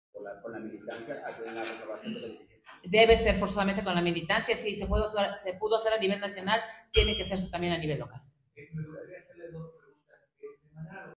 Militancia debe decidir sobre renovación de dirigencia del PAN Puebla: Adriana Dávila Fernández
Adriana Dávila, aspirante a dirigir el Comité Ejecutivo Nacional (CEN) del PAN señaló que en Puebla y los demás estados que renueven su dirigencia se debe de consultar a la militancia.